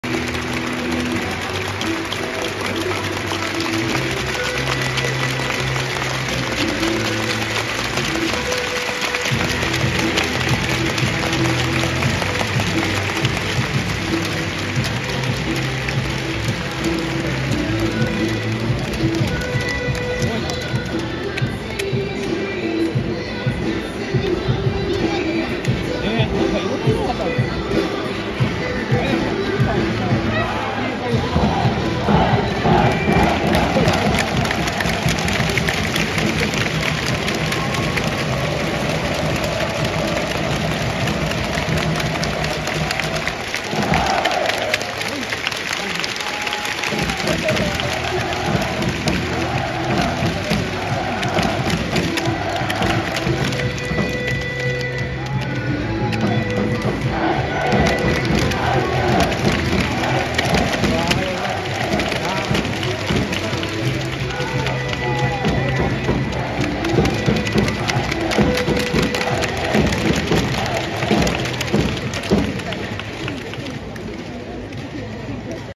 221016 J2第41節 岡山VS秋田 GKウォーミングアップ開始
2022年10月16日　J2第41節
「ファジアーノ岡山」VS「 ブラウブリッツ秋田」戦
GKウォーミングアップ開始

シティライトスタジアム
スコア：岡山　1対2　仙台
観　客：12,570人　今シーズン最高＆3回目の1万人超